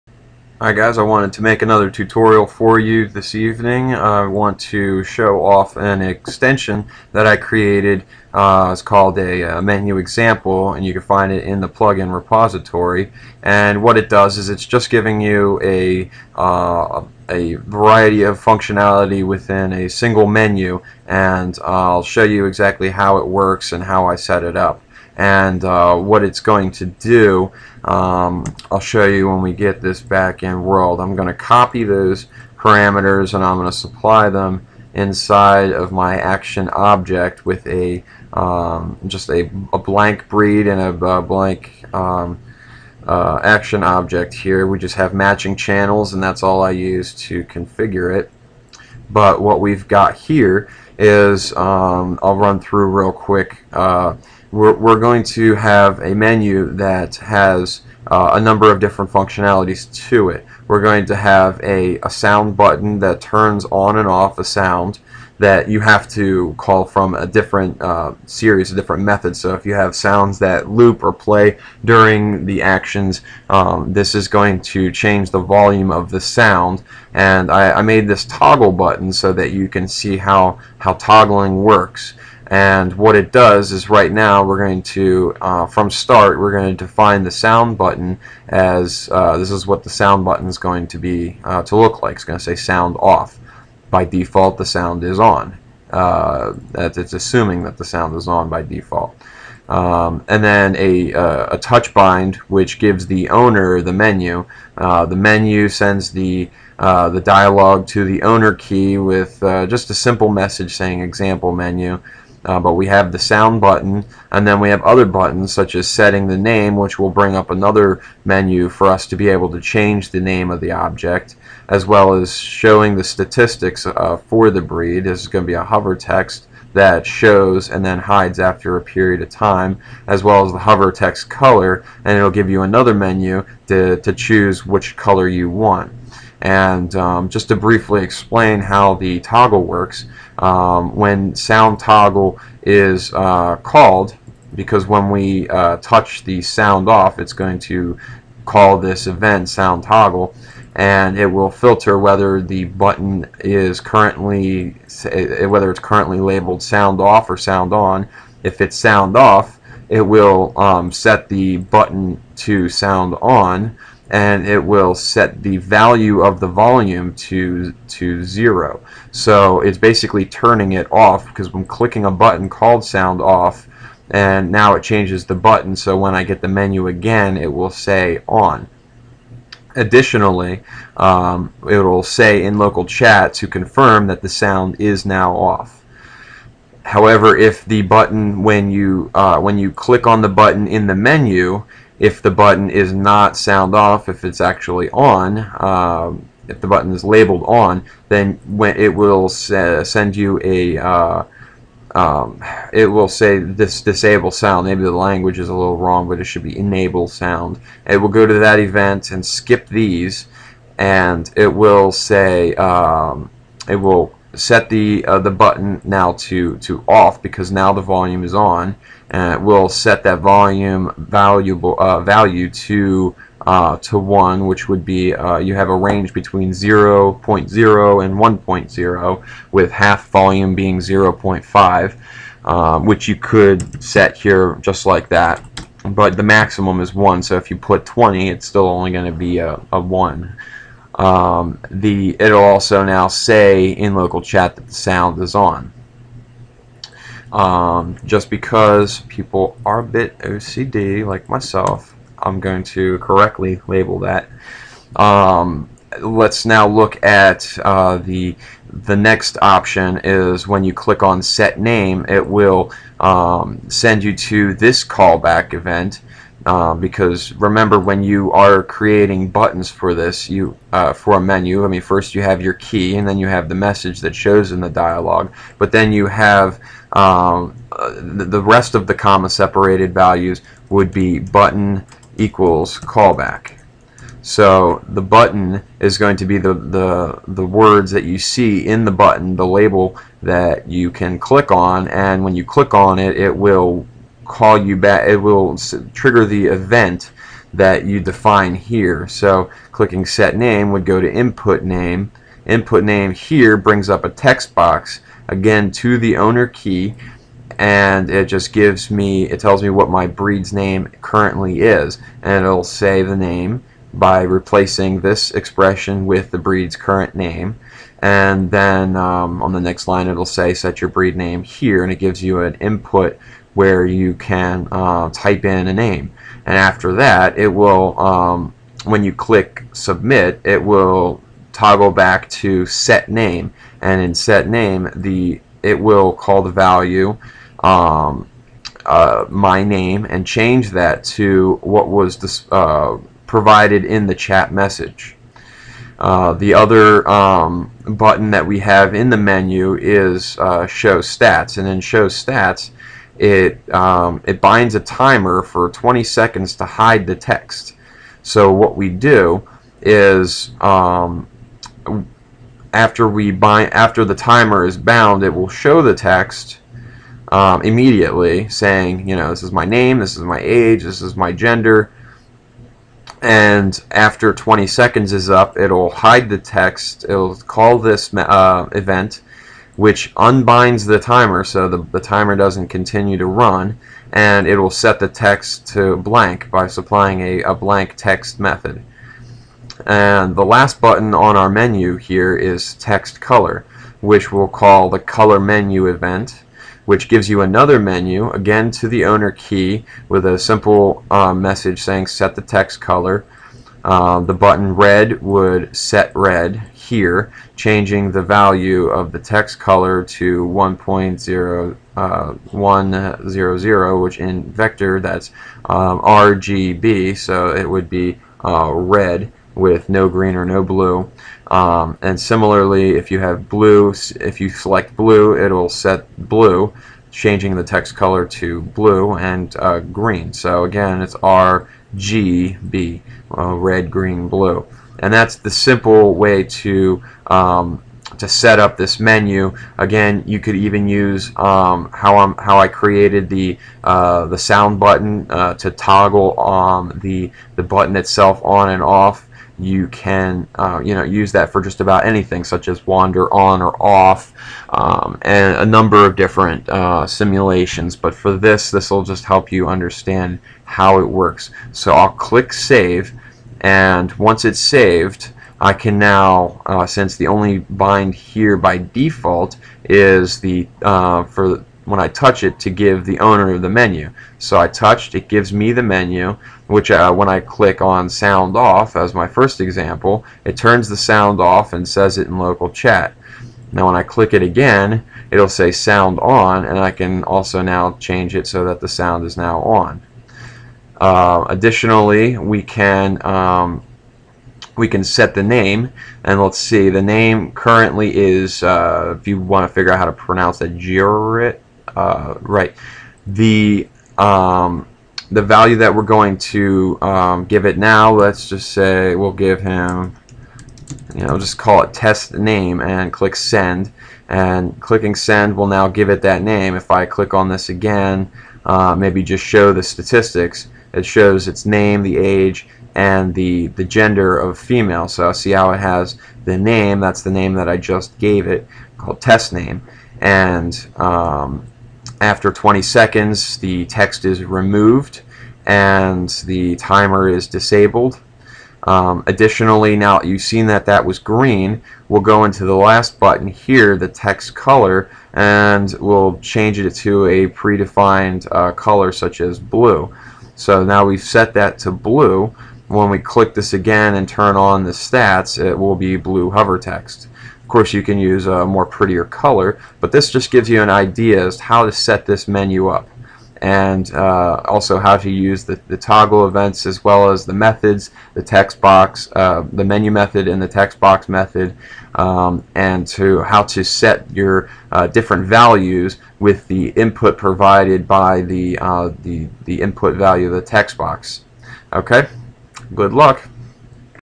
eco-breeds: Dynamic Menu Example
The eco stack provides a fast and concise LSL library that simplifies event handling, prim animations, and avatar interactions for rapid virtual pet development. This full featured LSL framework is lightweight and built with progressive enhancement, and has a flexible, easily scale-able design for adding features to physical and non-physical animals, pets, and other creations.